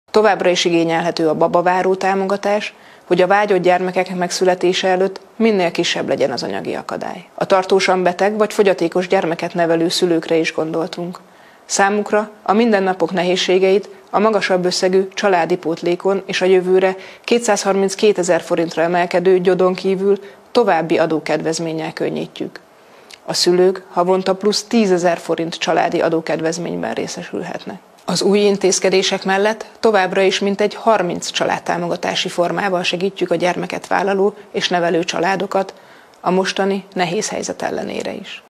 Új támogatással segítik a gyermeket nevelőket
A kormány az intézkedéseivel kiszámíthatóságot szeretne biztosítani a családoknak, mert ez segíti a gyerekvállalást - mondta a Kulturális és Innovációs Minisztérium családokért felelős államtitkára kedden. Hornung Ágnes az új családpolitikai intézkedéseket ismertetve beszélt a 30 év alatti anyák szja-mentességéről, a tanulmányaik alatt vagy azok befejezését követő két éven belül gyereket vállaló, szintén 30 év alatti nők diákhitelének elengedéséről. A szintén két évvel meghosszabbított falusi csok igénybevételének feltételei sem változtak, csupán a településlista módosult. Hozzátette, a családi otthonteremtési kedvezmény segítségével az elmúlt években minden 4. család tudott új otthonba költözni. A kormány az intézkedéseivel az emberek teljes "életútját" szeretné lefedni, a támogatásokat a változó körülményekhez és az aktuális gazdasági környezethez igazítják - mondta az államtitkár.